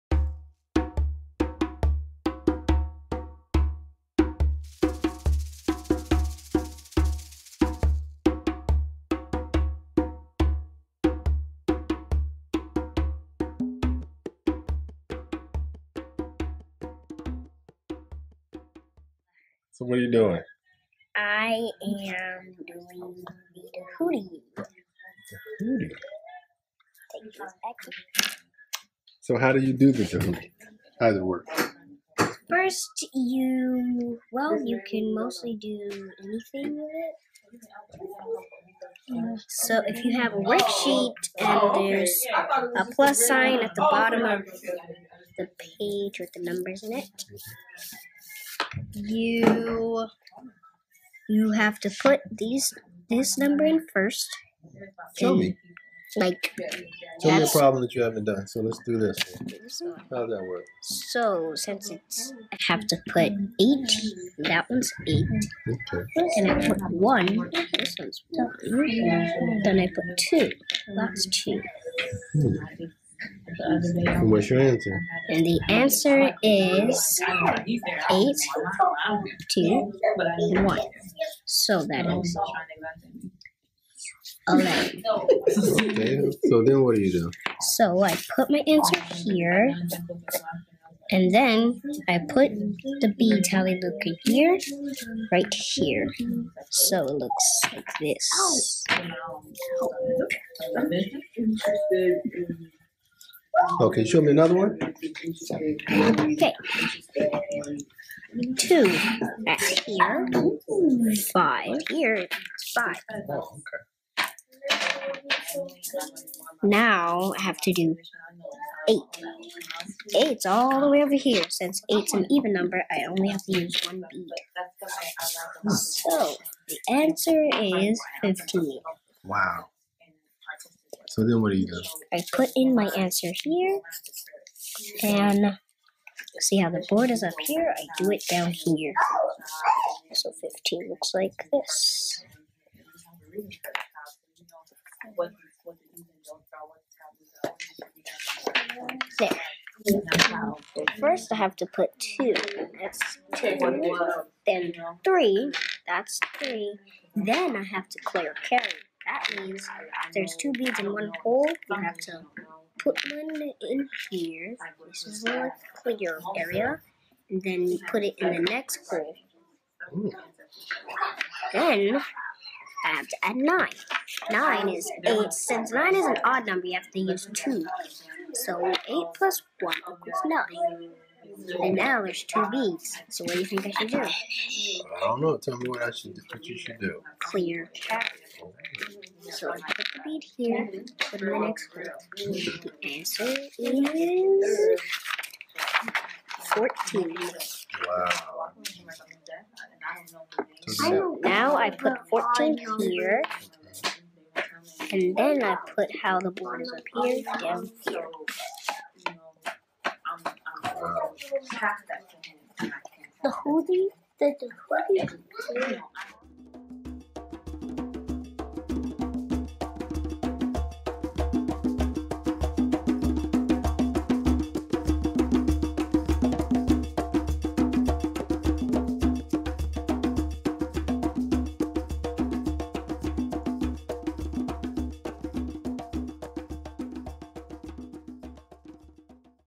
AFRICAN MATH™ 6/10
AFRICAN MATH™ Scribe demonstrates the use of the DHUTI (AFRICAN ABACUS™) on a basic addition worksheet.